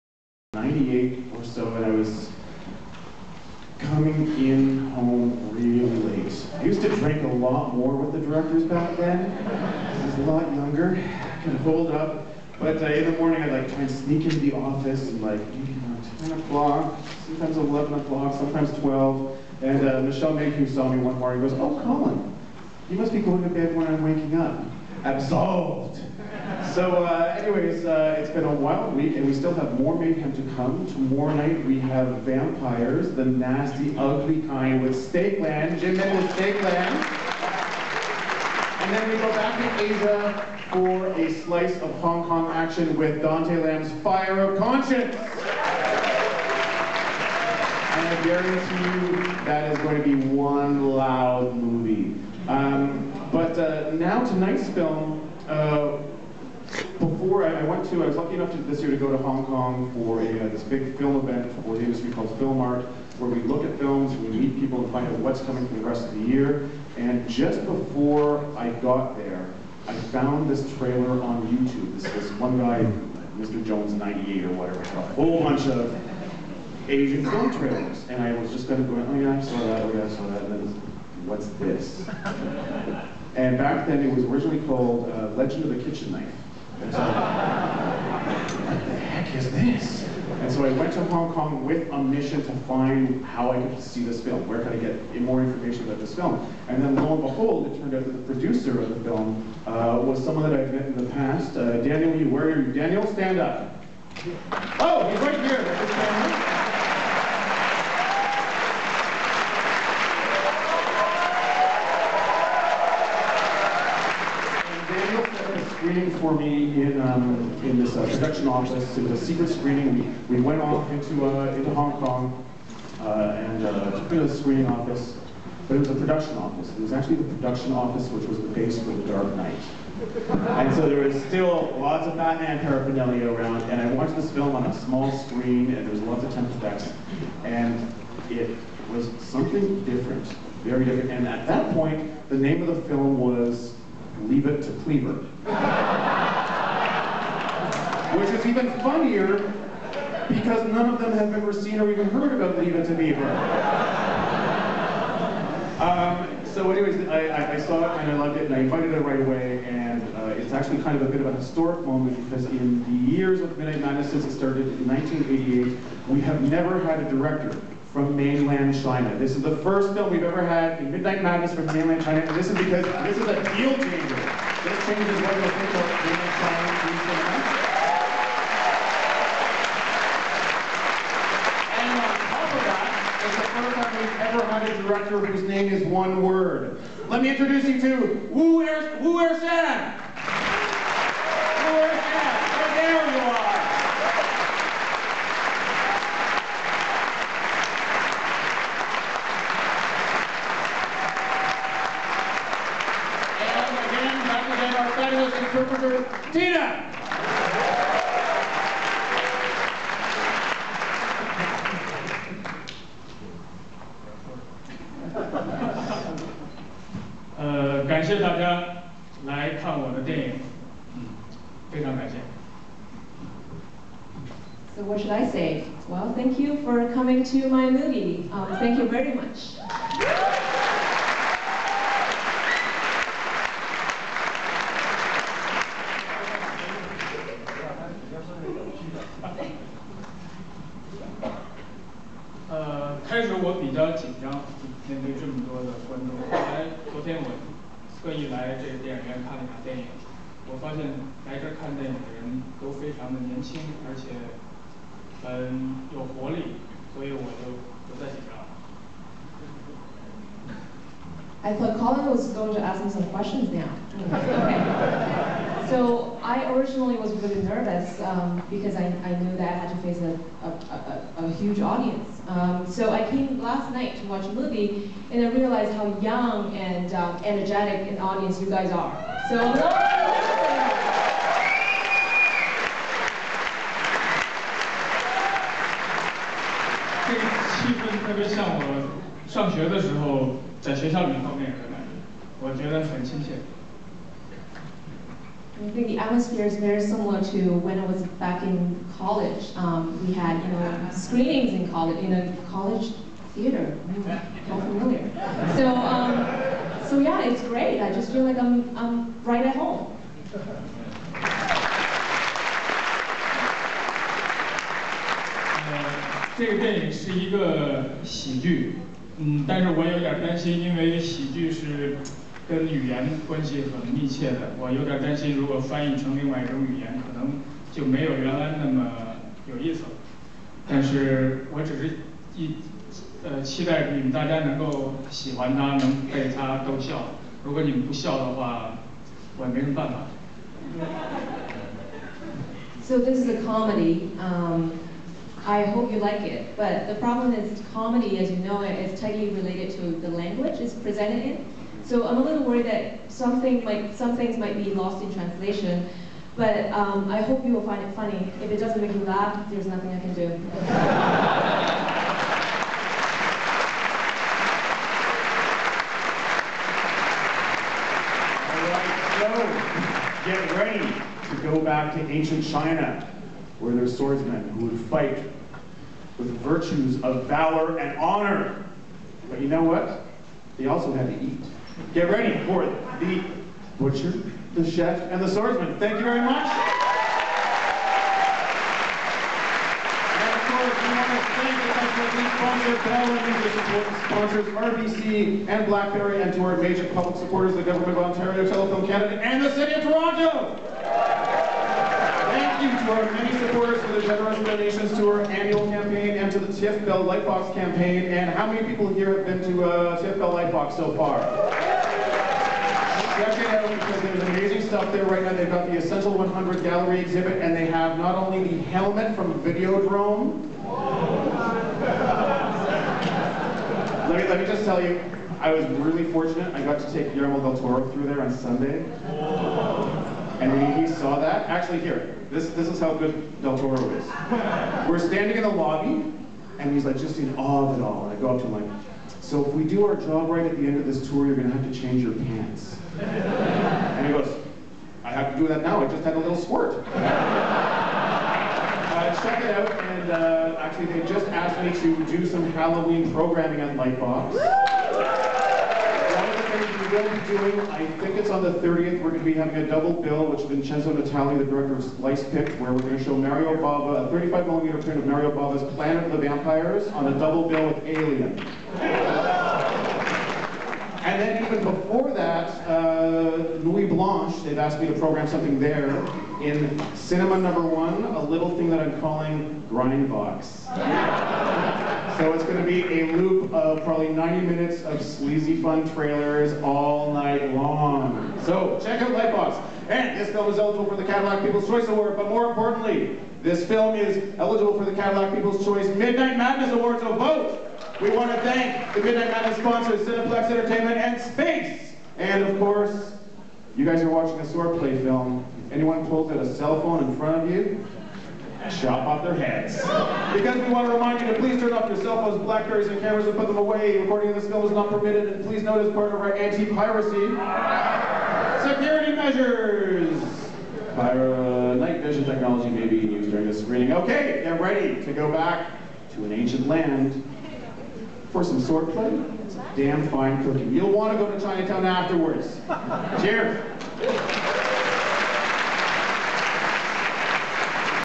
THE BUTCHER, THE CHEF AND THE SWORDSMAN (2010) introduction with director Wuershan at TIFF 2010
Introduction of director Wuershan -- the first mainland Chinese filmmaker ever to appear at a TIFF Midnight Madness screening -- at his gonzo period action-comedy THE BUTCHER, THE CHEF AND THE SWORDSMAN.